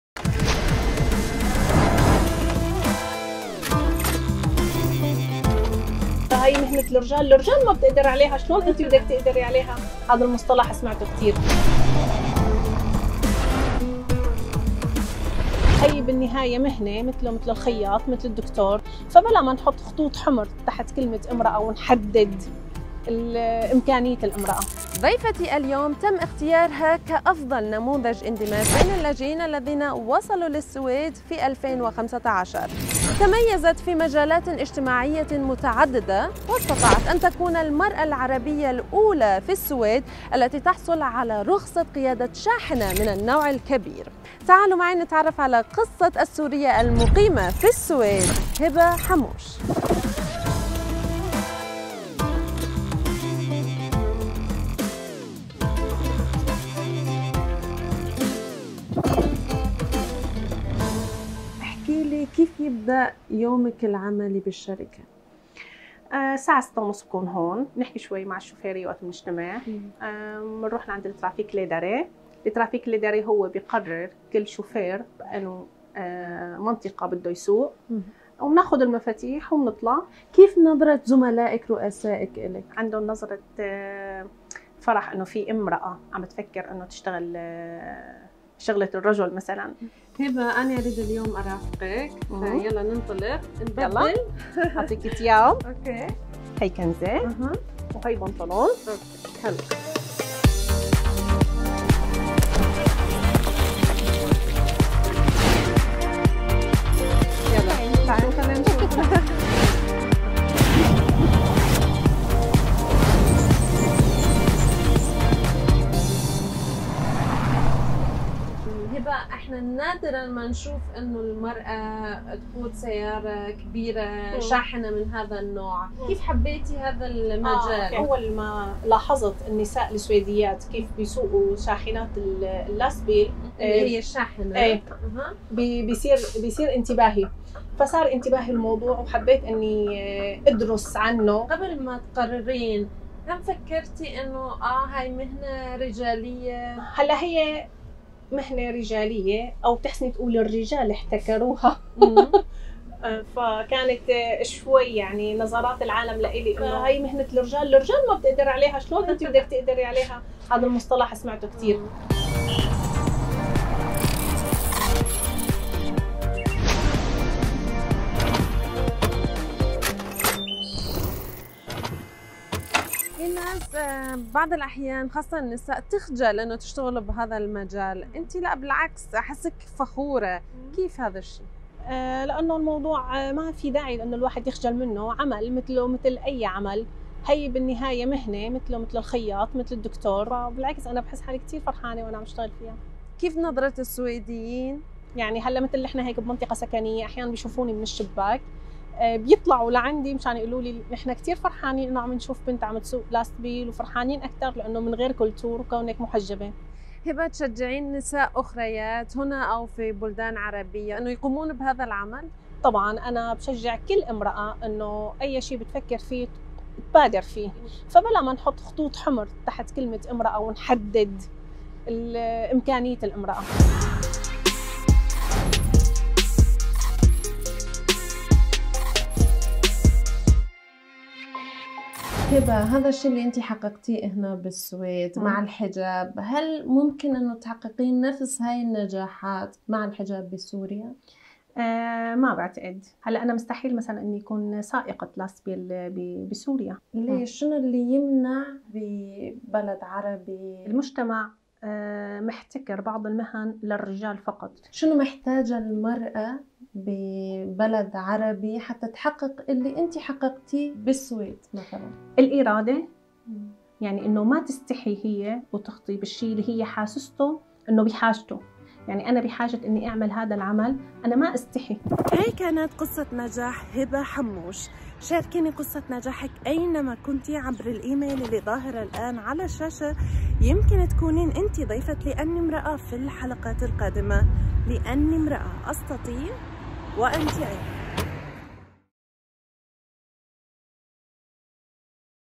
هبة حموش: قصة ملهمة لامرأة عربية في المهجر
هبة حموش لاجئة سورية تقتحم مهنة غير مألوفة، وتصبح المرأة العربية الأولى في السويد التي تحصل على رخصة قيادة شاحنة من النوع الكبير
الحلقة الأخيرة من الموسم الثاني من برنامج